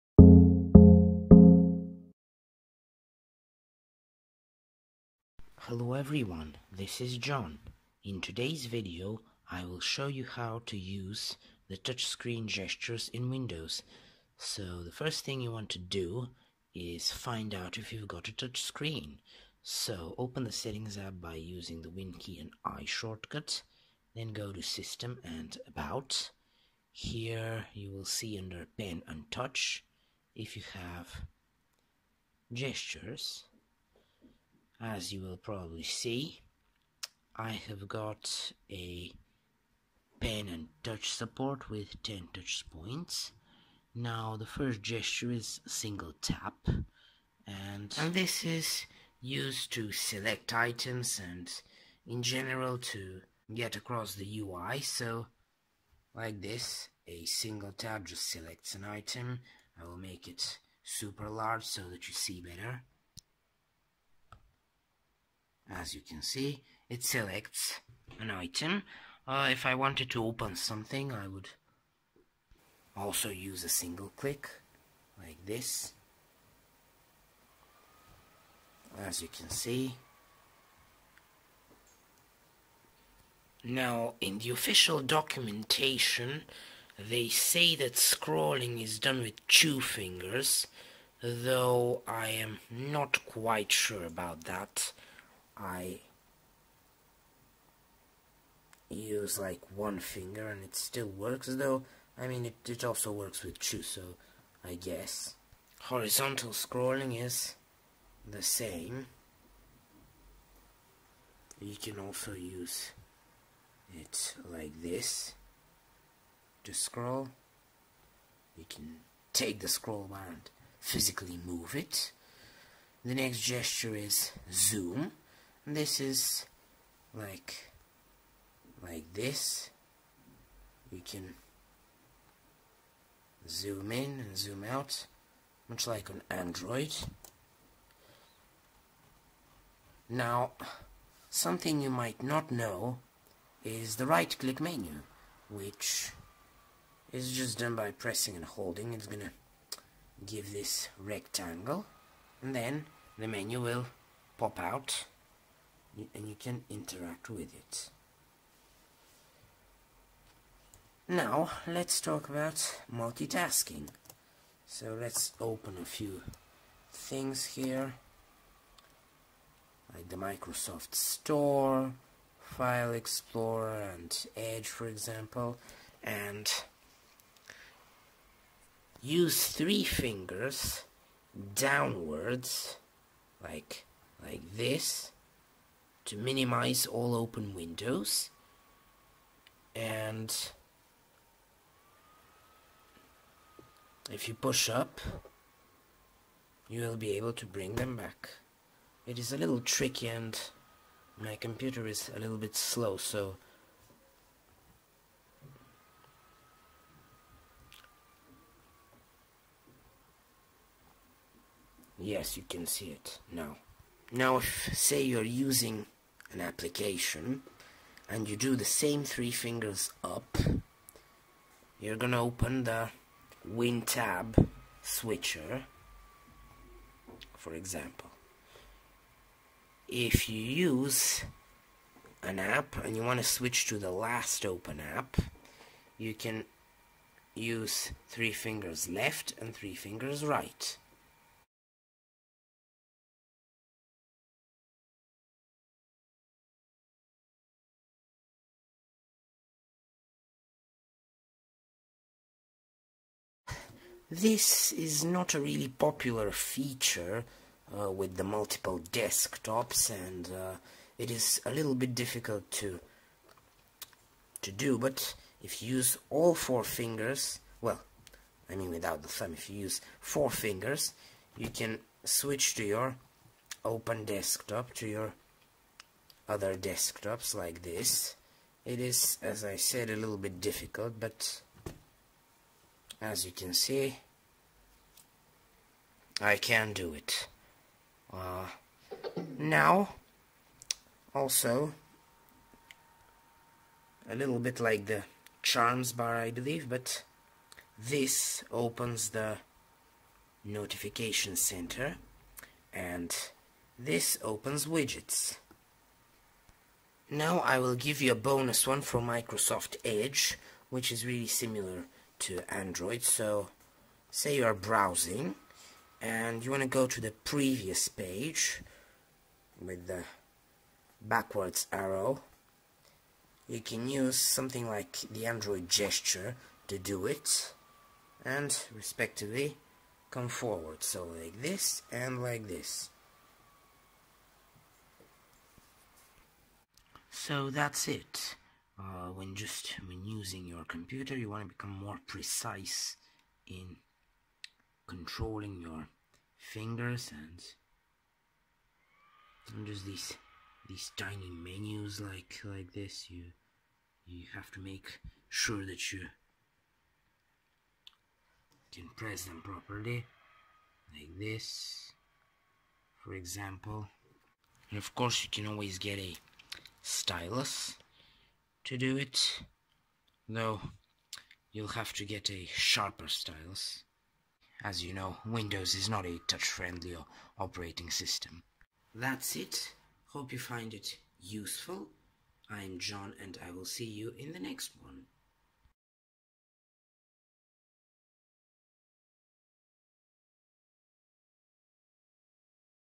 All touchscreen gestures in Windows 11 explained
Many laptops come with touch these days. Tablets and all-in-ones are becoming more popular and since 2012 with the release of Windows 8, Microsoft has made real efforts to improve the touch-readiness of Windows - first with the introduction of the Start screen and the Metro flavor of Windows. To the OS were then added many more touch improvements and while Windows 10 is mostly a regression since Microsoft returned the OS to desktops, Windows 11 has definitely improved a lot of core parts of the system. For example, the new context menus are much better for touch and File Explorer is back with a new ribbon interface. The Settings app has once again been revamped. Overall, Windows is not what it used to be before the smartphone revolution. And, even though many gestures are similar to mobile operating systems we are used to, I introduce some really handy gestures about multiple desktops and application switching, which are performed with three and four fingers.
By the way, who noticed my new profile picture?